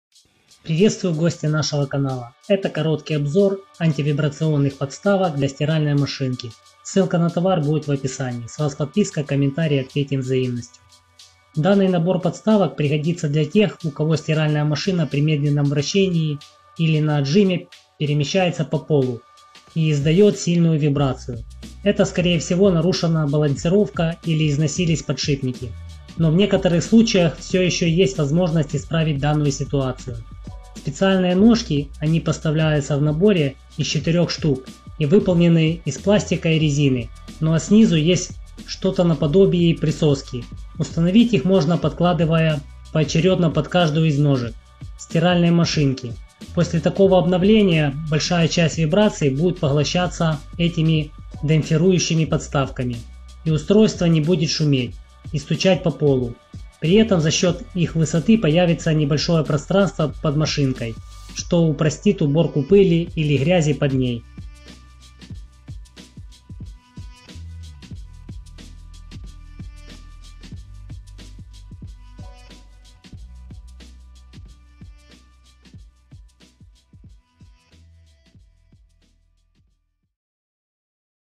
Антивибрационные подставки для стиральных машин: полезный товар с алиэкспресс
Набор антивибрационных подставок, для стиральных машин. Заказать
рекламодатель:

Получите купон на скидку 250 руб при заказе от 500 руб для всех покупателей СНГ (кроме Украины).